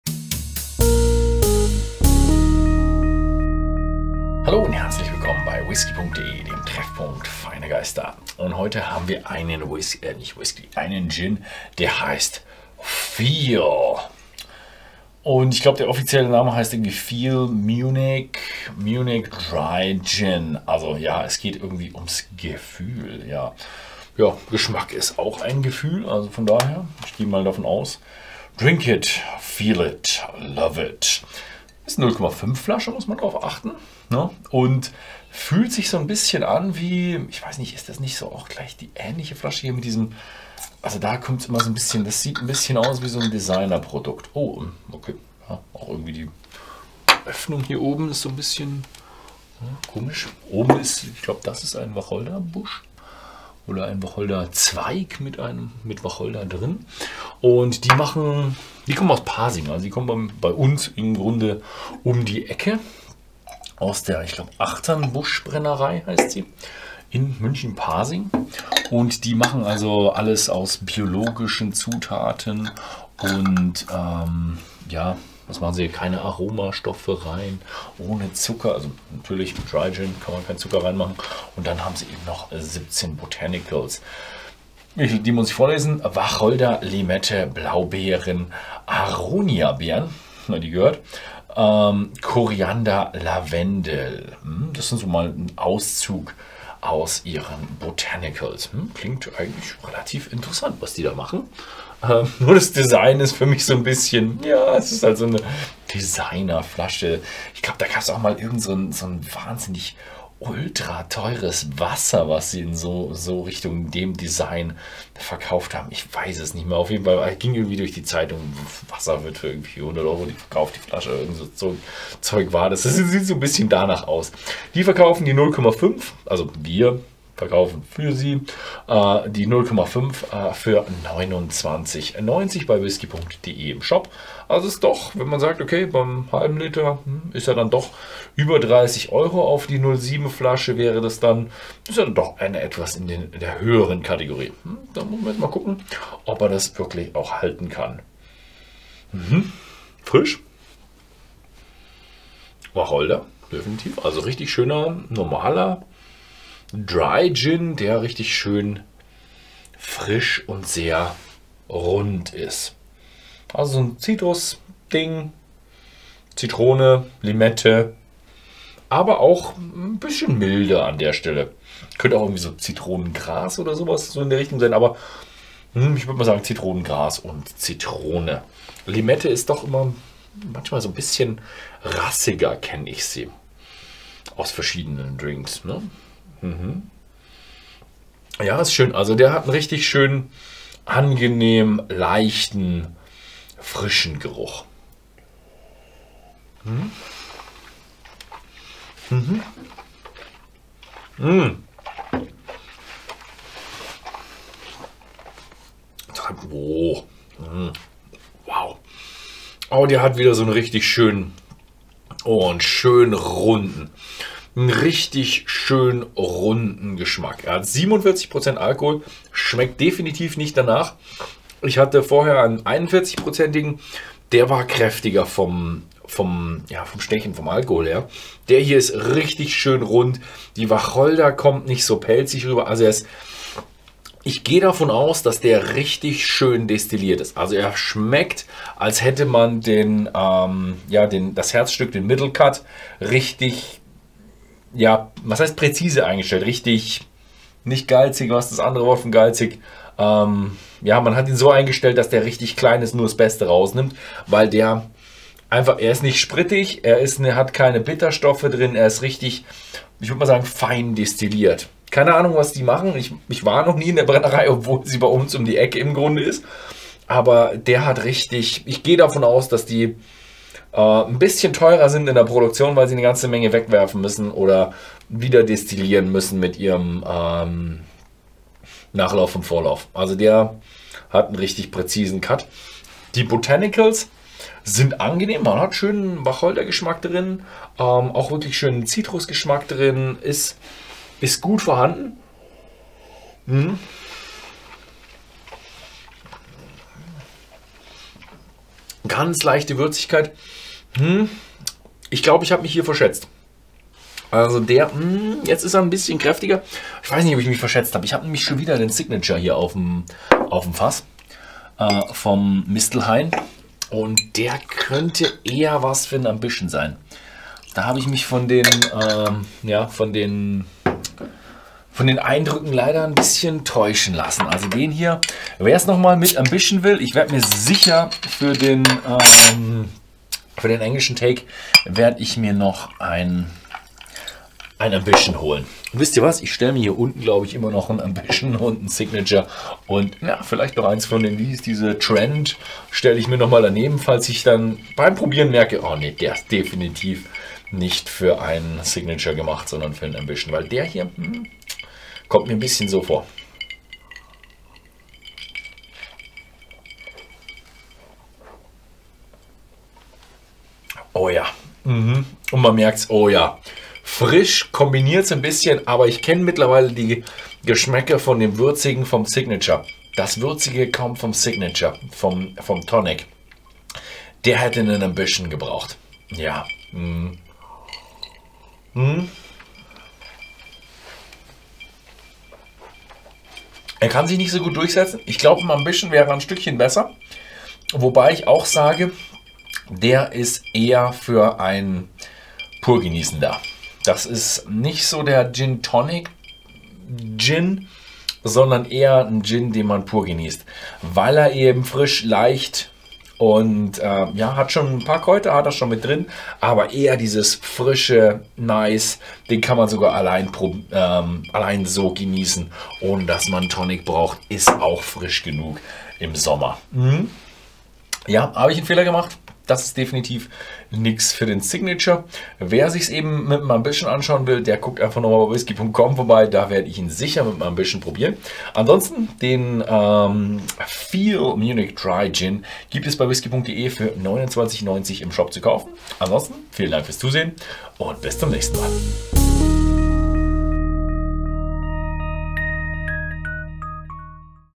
Feel! Munich Dry Gin | Gin Verkostung
Nosing 02:26
Wir verkosten den Feel! Munich Dry Gin. Der aus München stammende Feel! Dry Gin wird in kleinen Batches destilliert und besteht aus insgesamt 17 handverlesenen Botanicals. Die Grundlage bildet Wacholder, über den sich Limetten, Blaubeeren und Aroniabeeren entfalten. Die Würze durch Koriander wird von Lavendelnoten gemildert, was aus der schonenden, mehrfachen und langsamen Destillation resultiert.